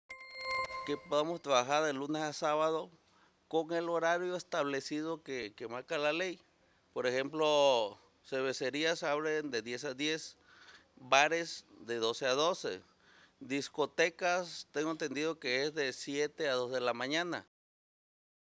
BITE 01 OPERACIÓN BARES
Bares, cantinas, antros y centros nocturnos de Salina Cruz han reactivado operaciones luego que el municipio pasará a semáforo naranja, desacuerdo a lo emitido por la Jurisdicción Sanitaria 02.
Filiberto Domínguez Clemente, presidente de la Asociación Ike Sidi Biá; dio a conocer que los horarios de operación varían dependiendo el giro comercial, donde los bares y cantinas cuentan con jornadas de 12 horas y los antros o centros nocturnos, de 7 horas.